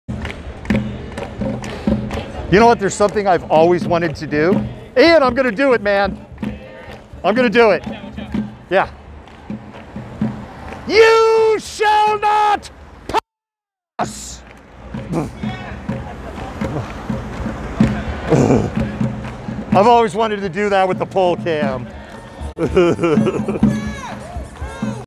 Highlight: LIVE! Seattle Anarchist Jurisdiction Day 129 - BLM Protests - The Malcontent Goes Gandalf
I always wanted to do this, and finally had an opportunity to do it.